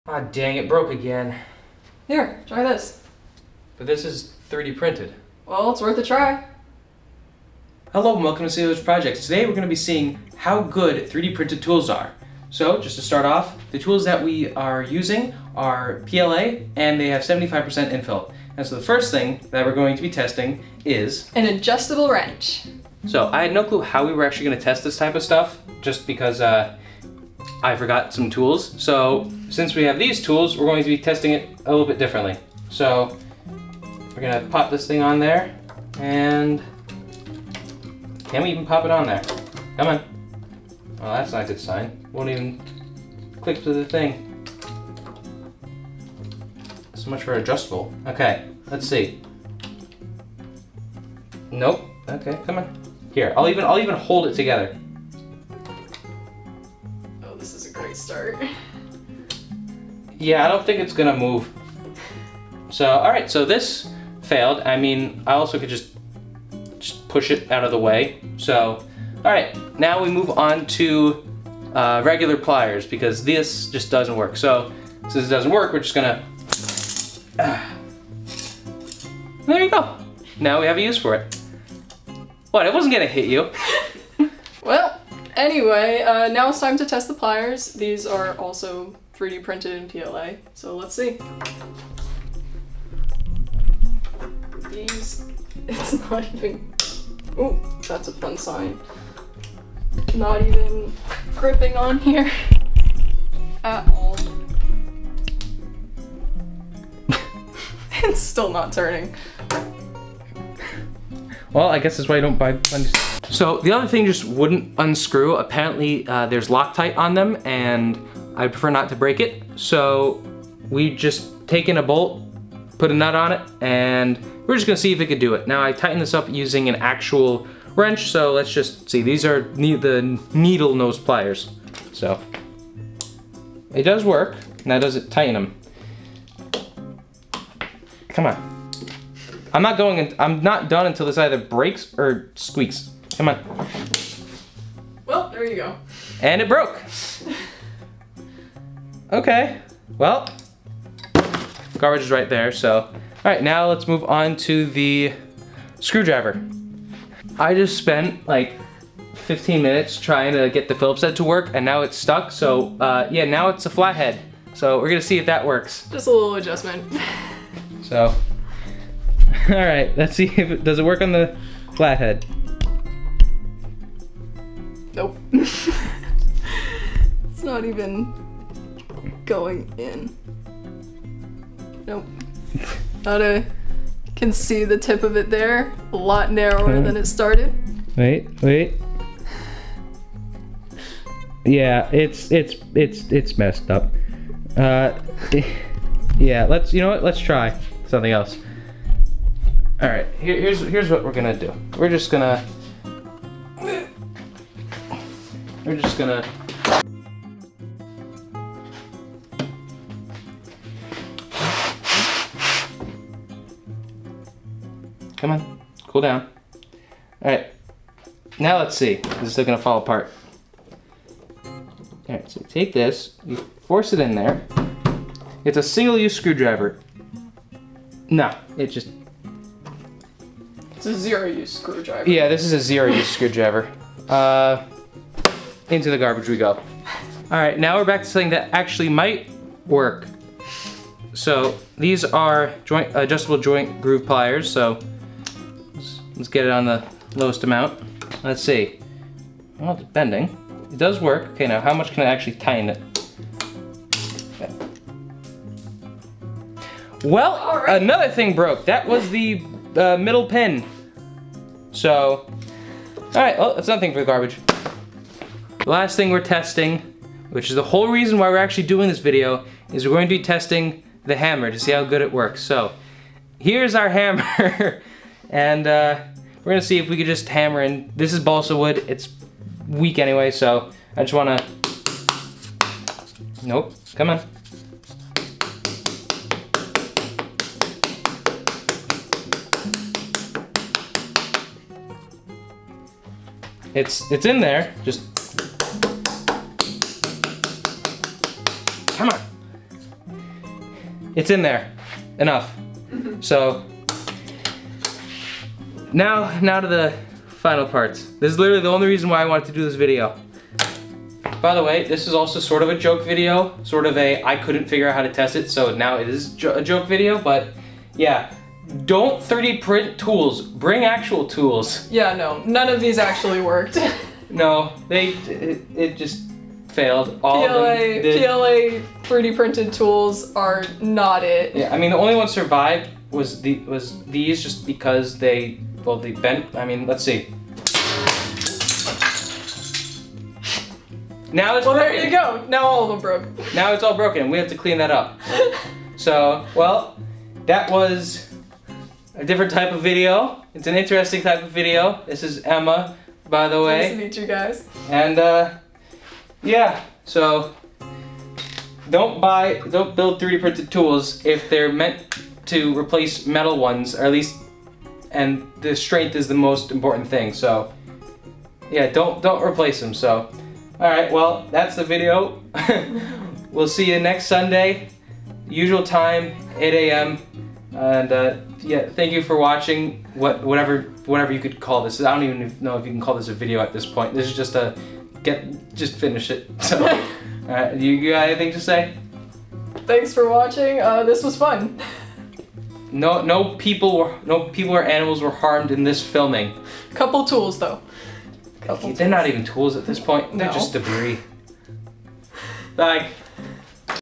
How good are 3D printed tools
In this video Emma and I go and test 3D printed tools! (I forgot to bring the stuff to test them so this is more fun than scientific!)

R3